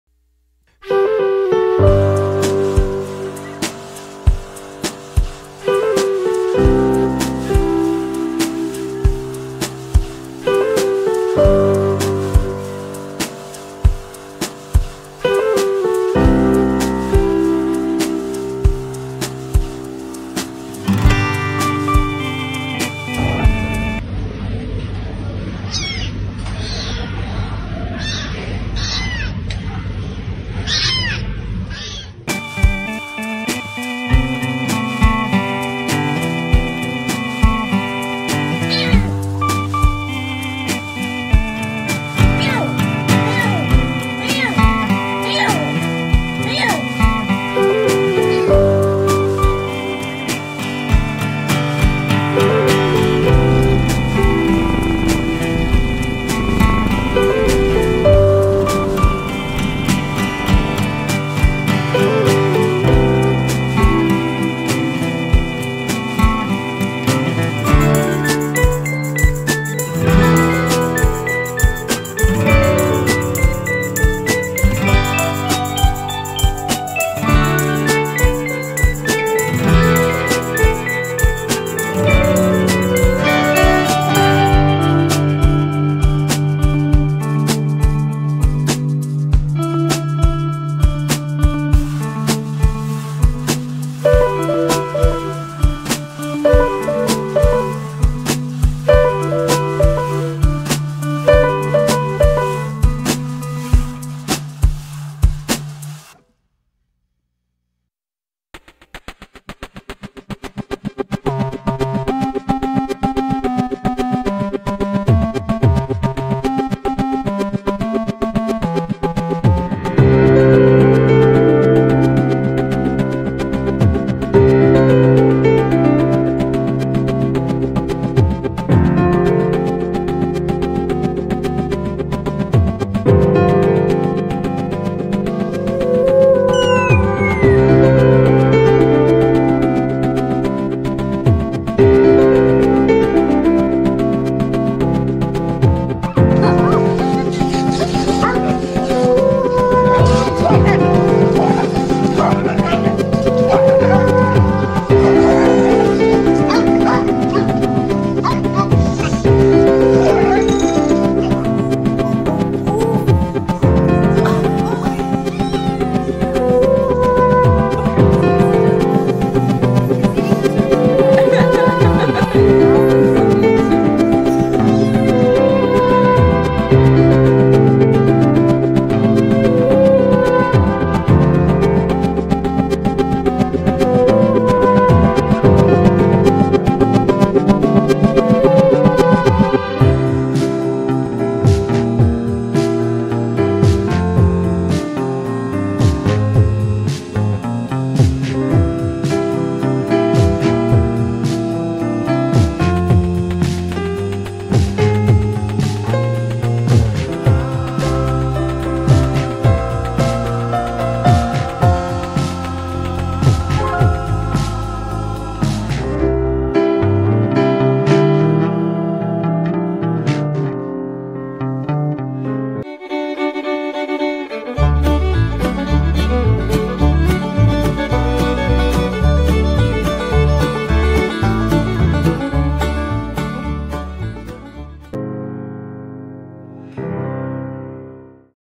Adorable Cattos and Kittos
Fun mix of cat and kitten videos. Not for children but nothing offensive.